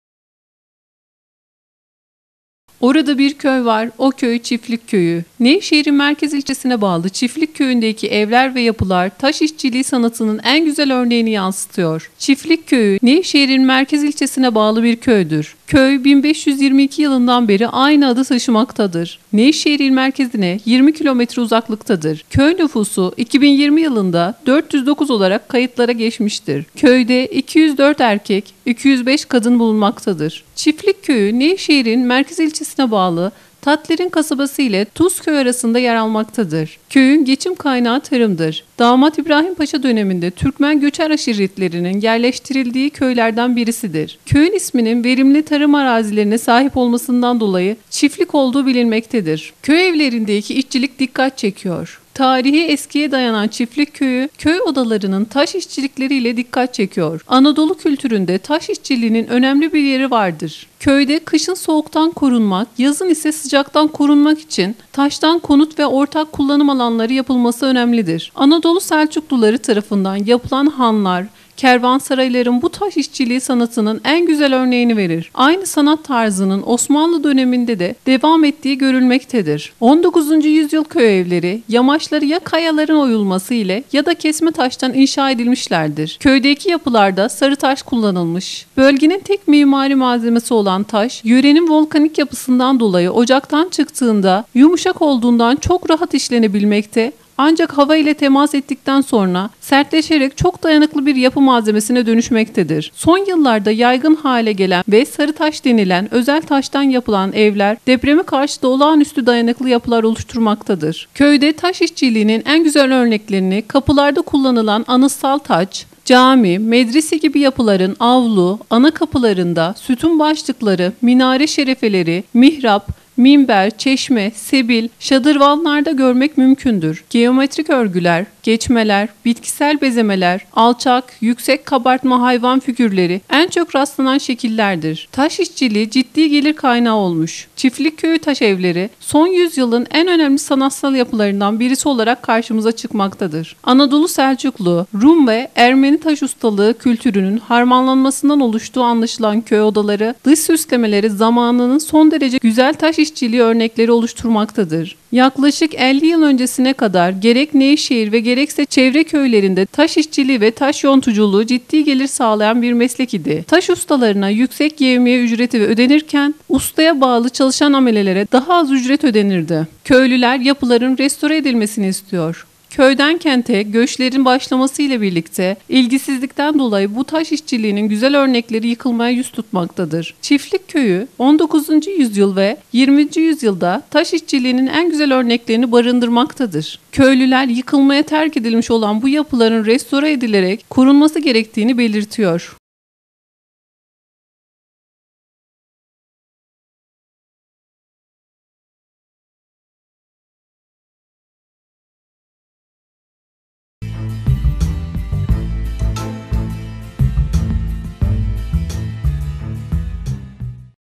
NEVŞEHİR-ÇİFTLİK KÖYÜNÜN BİLİNMEYENLERİ
Çiftlik Köyü,  Nevşehir’in merkez ilçesine bağlı bir köydür. Köy 1522 yılından beri aynı adı taşımaktadır.